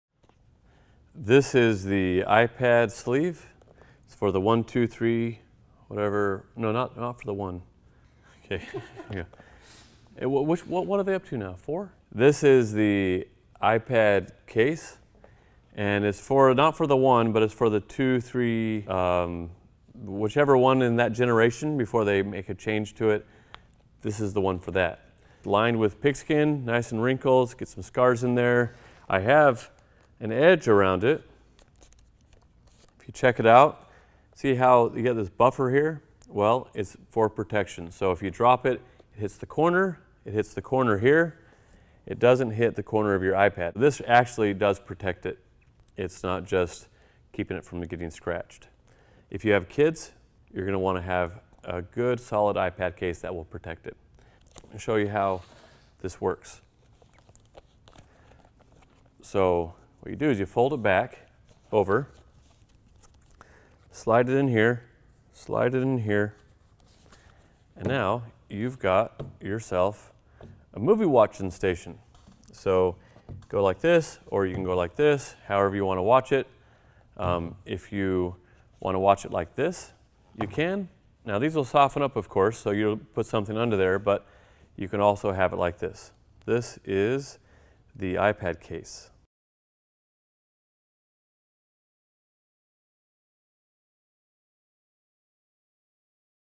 iPad Case, Protector of the iPad 2, 3, 4 and More
Not only does this greatly protect against shock, but it can stand up the iPad in several different configurations. This cover protects the iPad 2, 3, and 4. It's so well designed, it will out last iPad 5, 6, 7 and you get the picture.